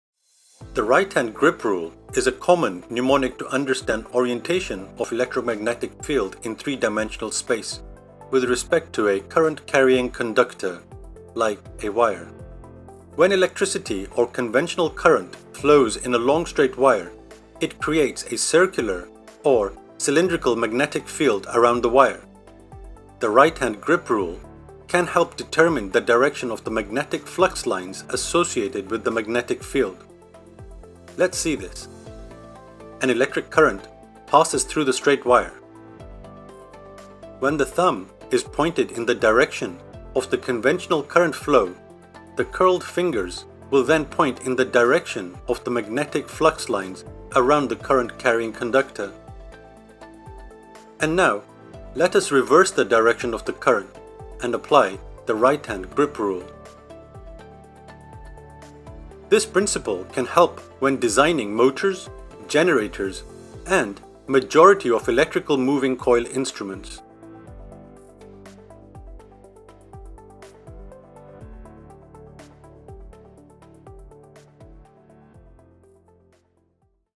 What Is The Right Hand Grip Rule?
An introduction using and applying the right hand grip rule. The right-hand grip rile can be used to understand the orientation of the induced electromagnetic field in three dimensional space with respect to a current carrying wire. This relates to electromagnetism and can help when designing motors, generators and majority of electrical moving coil instruments.